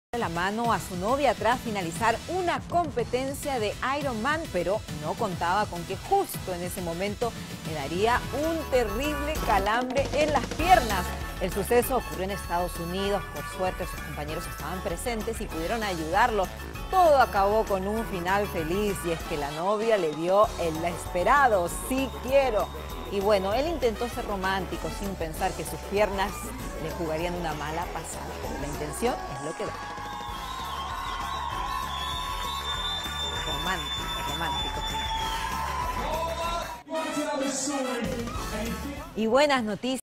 Estados Unidos: Joven sufre calambre mientras pedía matrimonio a su pareja
Lo que iba a ser un momento crucial en su vida terminó con una anécdota muy ocurrente. En los Estados Unidos, un joven deportista sufrió calambres en ambas piernas en el preciso momento en el que le estaba pidiendo matrimonio a su pareja.

El deportista en mención había terminado de participar en una competencia de Ironman, cuando procedió a inclinarse para pedirle la mano a su pareja, no obstante, la actividad que realizó previamente le jugó una mala pasada y terminó acalambrado en ambas piernas.

Final feliz

Rápidamente, sus compañeros se aproximaron para atenderlo, no obstante, en medio del cansancio y el dolor del calambre, el deportista continuó con la pedida de mano y le mostró a su novia el anillo de compromiso. Afortunadamente, todo acabó con final feliz luego que la novia le diera el tan esperado "sí, acepto".